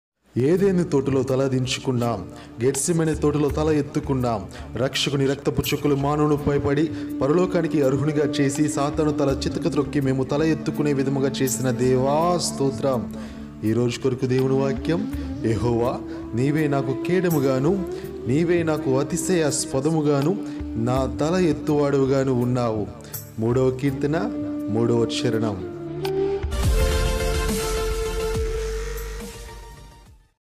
ఈరోజు మన కొరకు దేవుని వాక్యం ఇదిగో...| ATHMEEYA YATHRA
ఏదోను తోటలో తల దించుకున్నాం...
గెత్సేమనే తోటలో తల ఎత్తుకున్నాం
రక్షకుని రక్తపు చుక్కలు మానవునిపై పడి
పరలోకనికి అర్హునిగా చేసి సాతాను తల చితక త్రొక్కి
మేము తల ఎత్తుకునే విధముగా చేసిన దేవా స్తోత్రం...